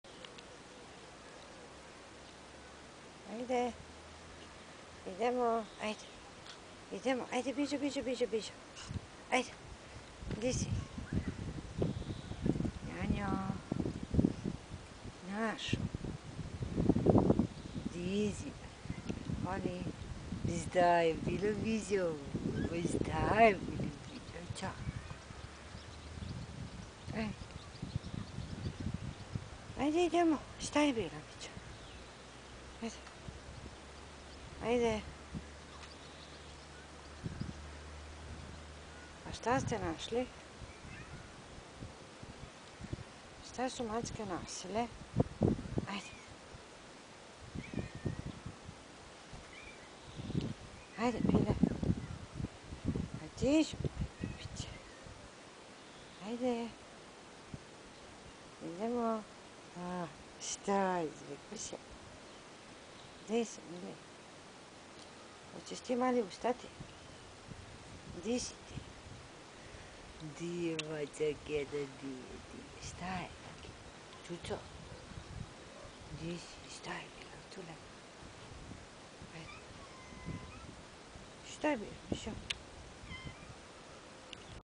Walking through the backyard
These are the kitties from Cat Shelter Felix.